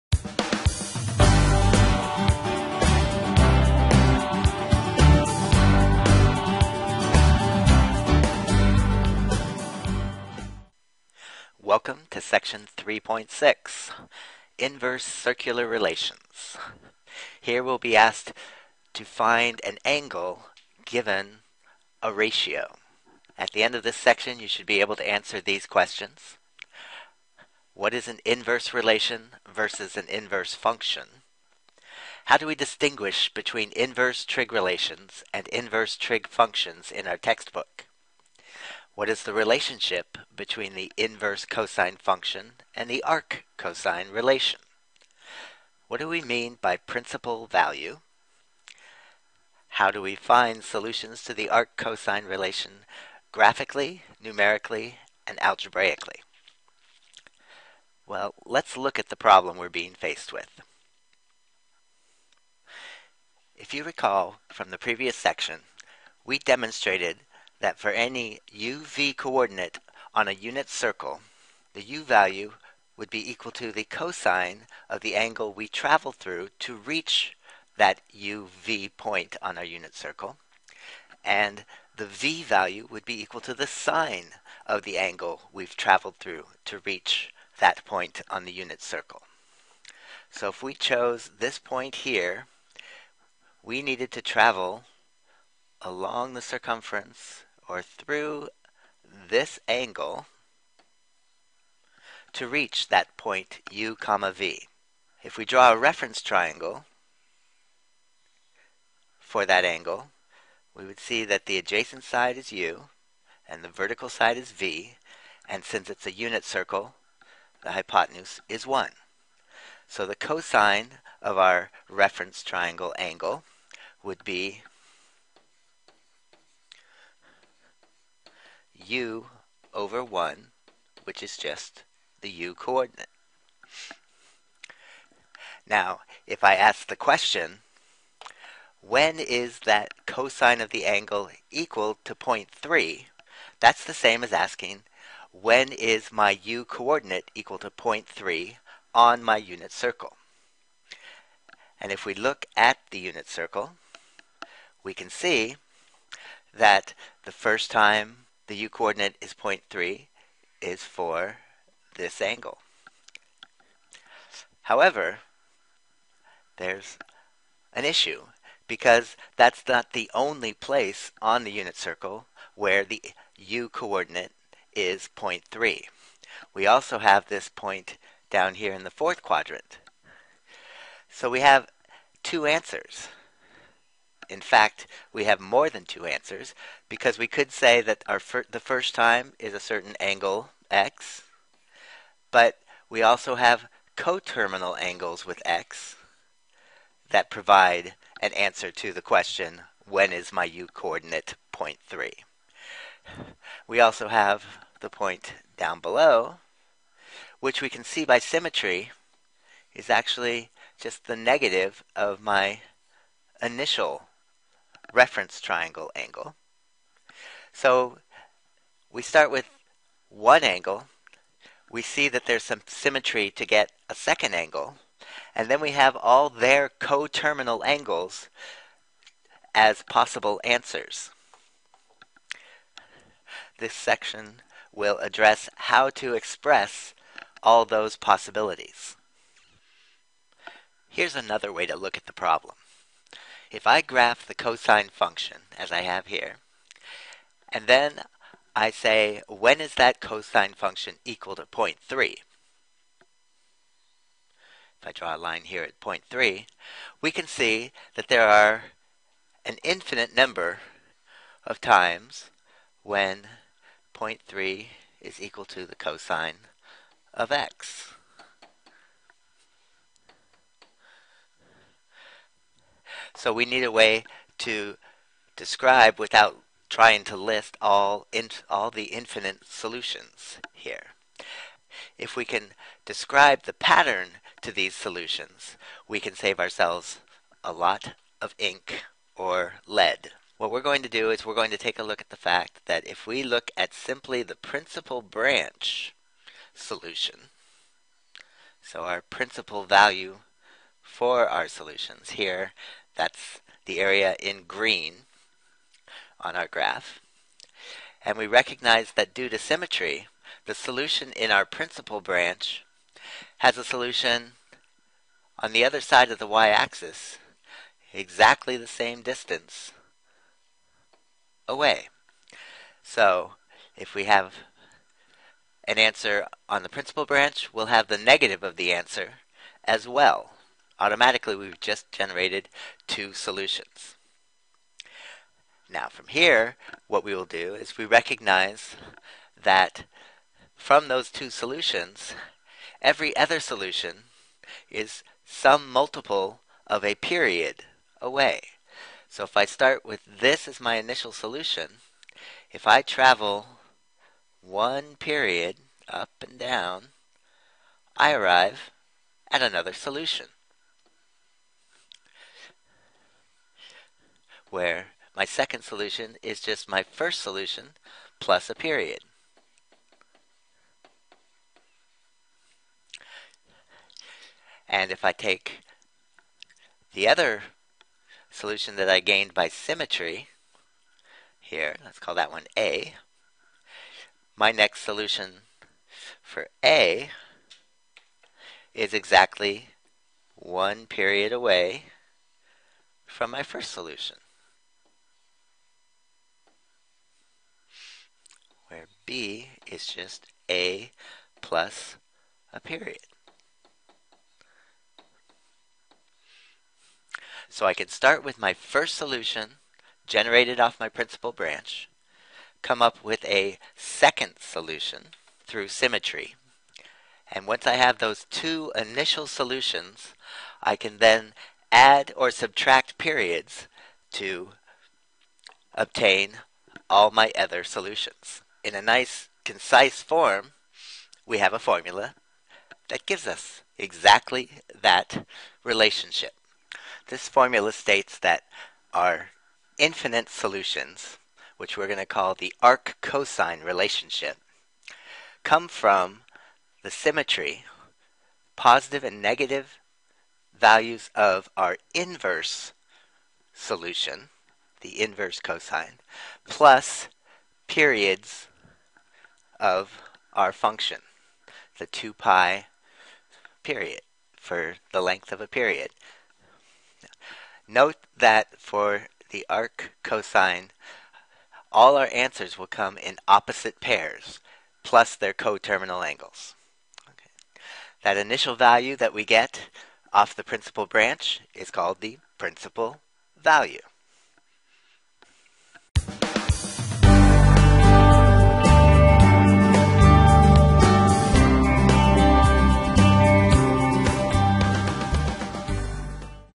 Lesson 3-06: Inverse Circular Relations (Given y, find x)
A discussion of inverse trigonometric relations with emphasis on the inverse cosine